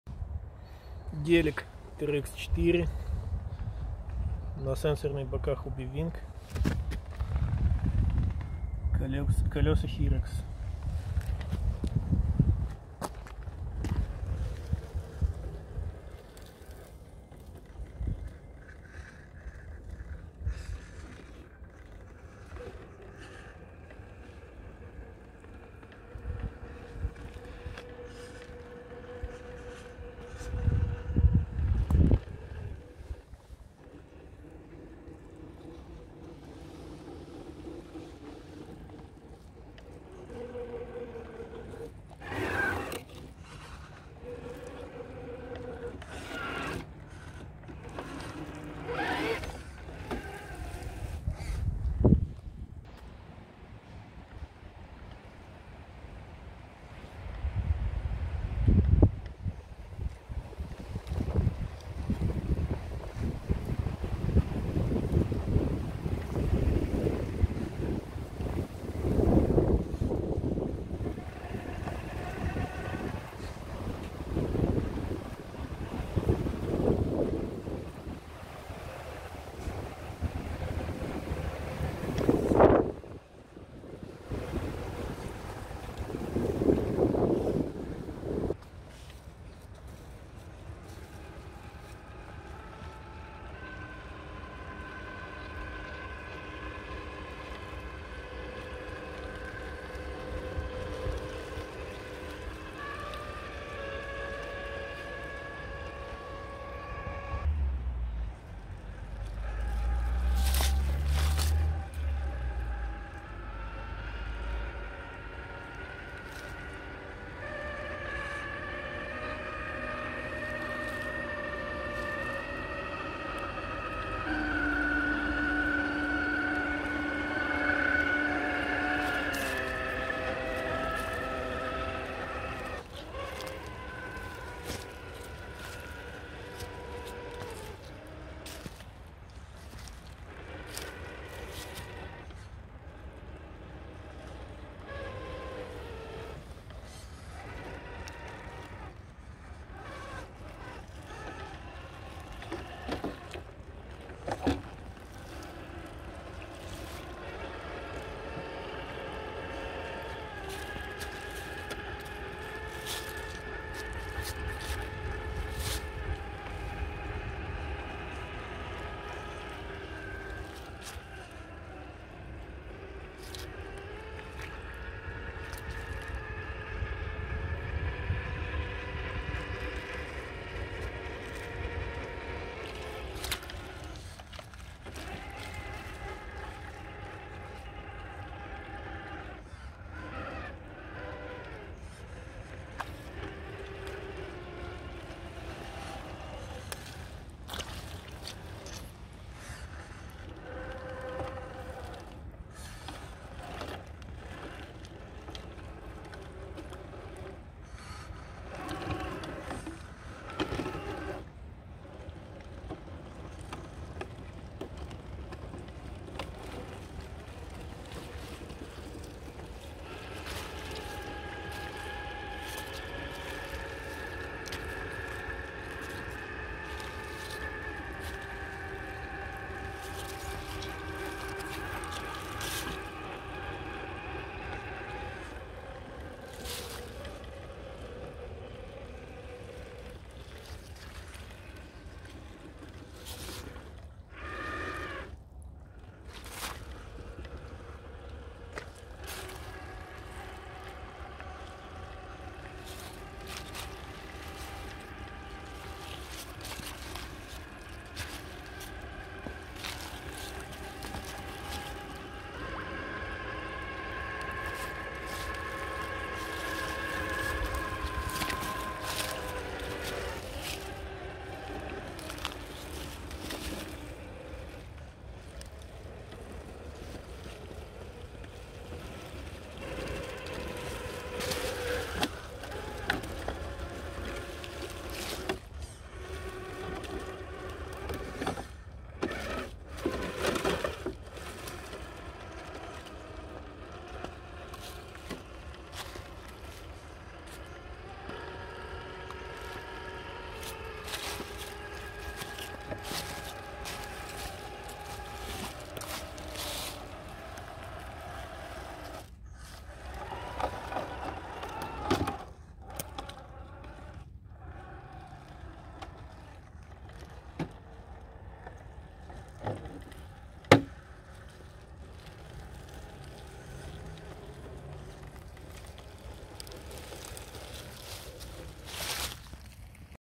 G500 trx4 and Hyrax by Proline test with sensored hobbywing in mud
testing hyrax predator by proline on my TRX4